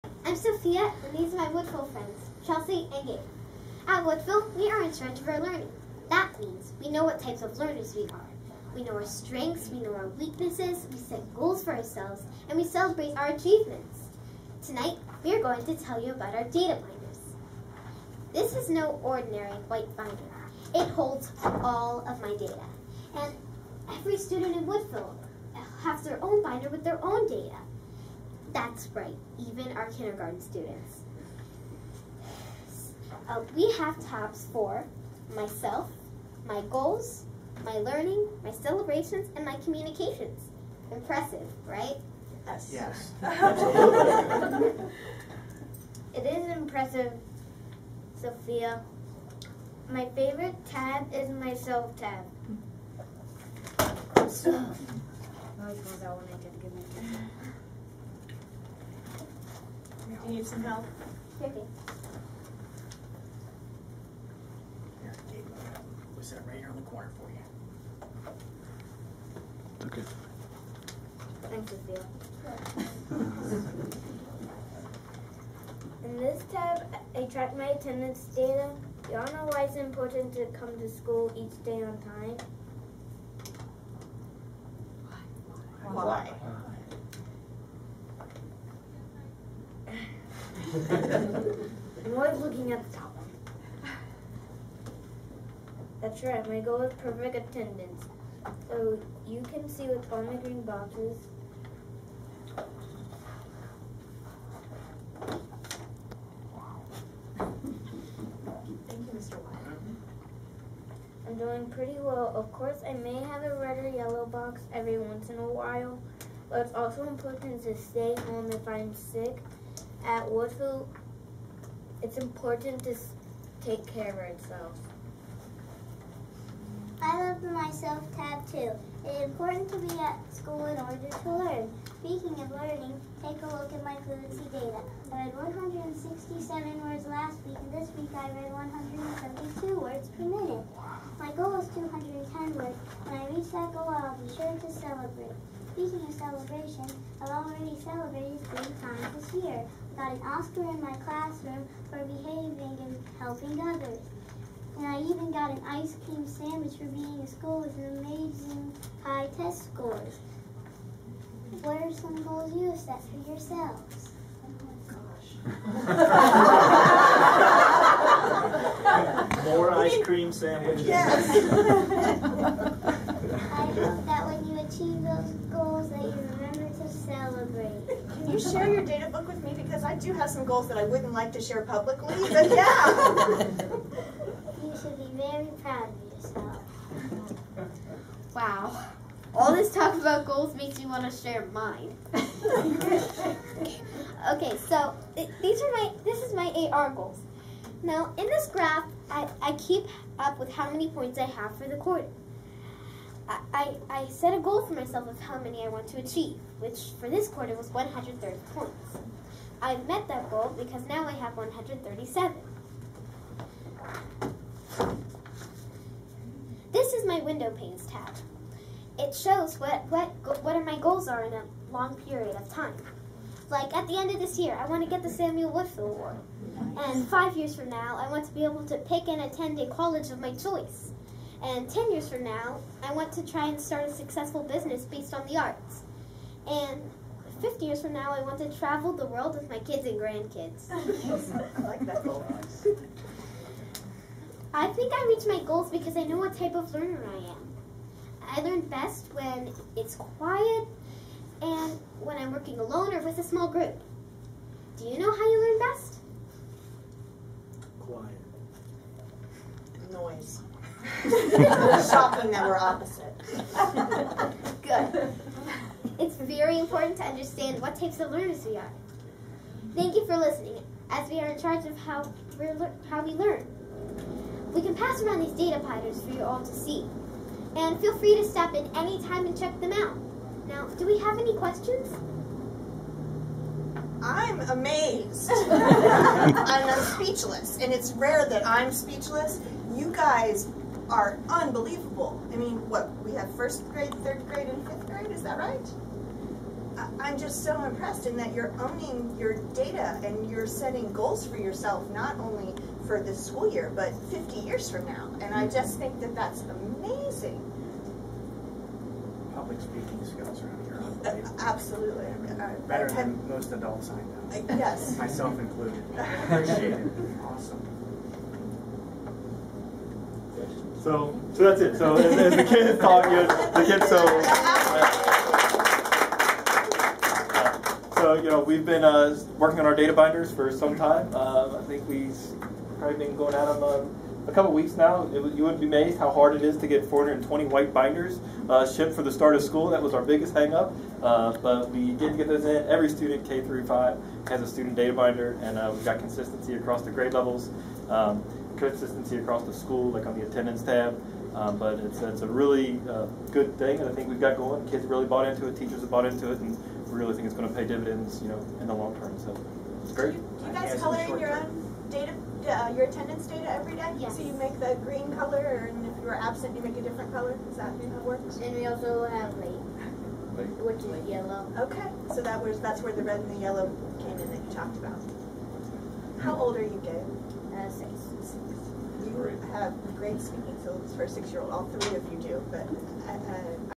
Woodfill Elementary School Data Binders initiative
Woodfill Elementary School Data Binders initiative presented at Fort Thomas School Board October Meeting by Woodfill Elementary School students Gabe Williams, Chelsea Crisler and Sophia Veshapidze